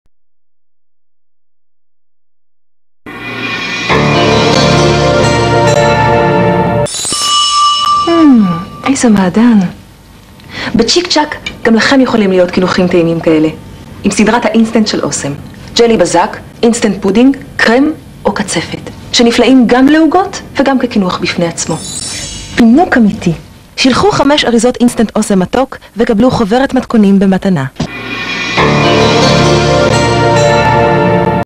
הפסקת פרסומות - אסם קינוחים ומנות אחרונות - ערוץ 2 - שידורי רשת - 20/08/1995 - סרטון #1174
בסרט ים של אהבה - ערוץ 2 - שידורי רשת - 20/08/1995
טיפ מהמטבח - אסם קינוחים ומנות אחרונות

בברכה,
אבי עמיצור.